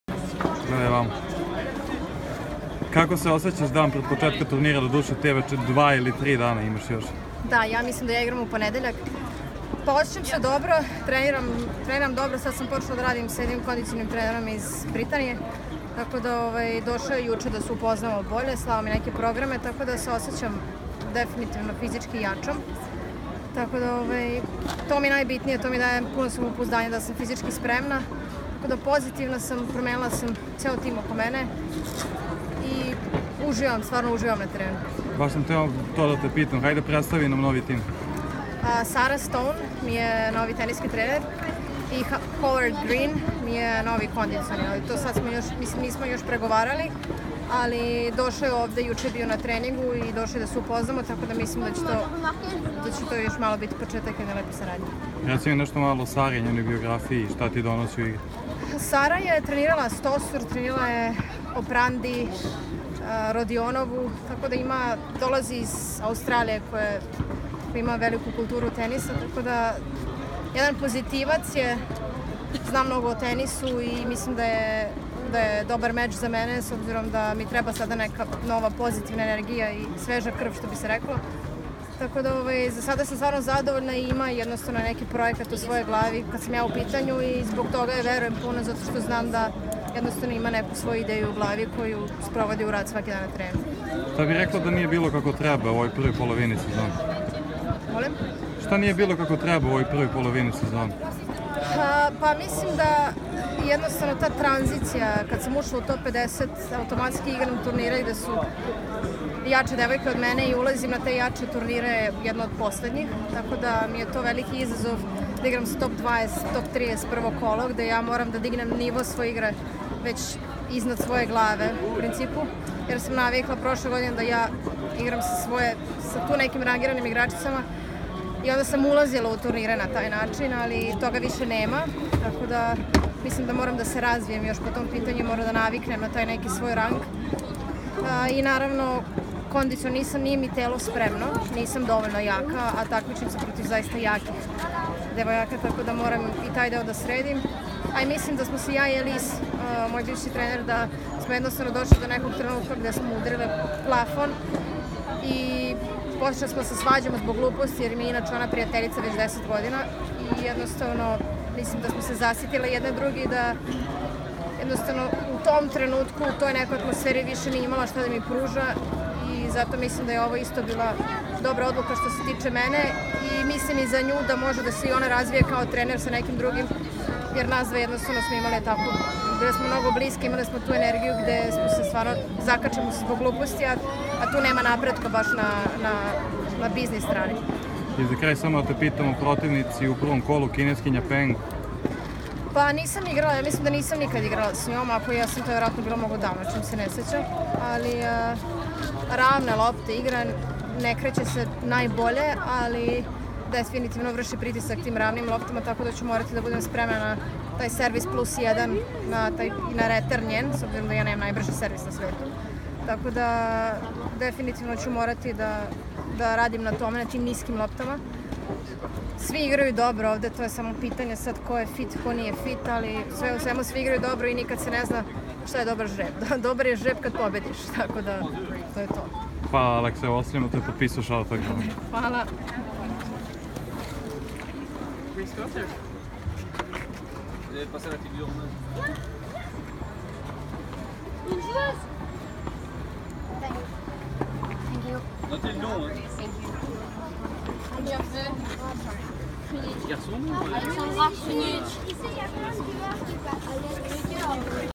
Aleksandra Krunić: Dobar je žreb kada pobediš | SPORT KLUB Tenis
Najbolja srpska teniserka Aleksandra Krunić rekla je u ekskluzivnom intervjuu za Sport Klub da imena protivnica nisu važna, već samo da li dobijete meč. Pred početak Rolan Garosa, Krunićeva se nada da će opravdati epitet teniserke koja uvek najbolje partije pruža na najvećim turnirima